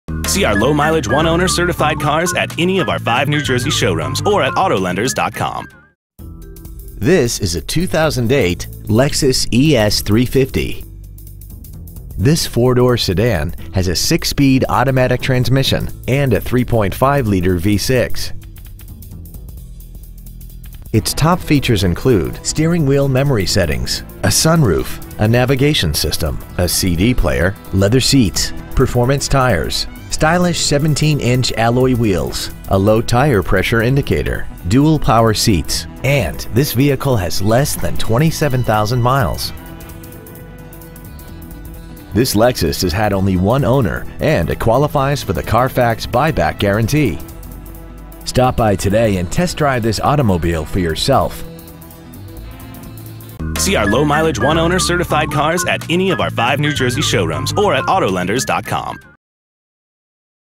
Used 2008 Lexus ES 350 Egg Harbor Township NJ
Year : 2008

 Make : Lexus

 Model : ES 350

 Engine : Gas V6 3.5L/210

 Trans . : Automatic

 Exterior : Moon Shell Mica

 Miles : 26,114

 Interior : Gray Leather

 Stock : L159828

 Located in Egg Harbor Township .

 Call to confirm the location .

 Auto Lenders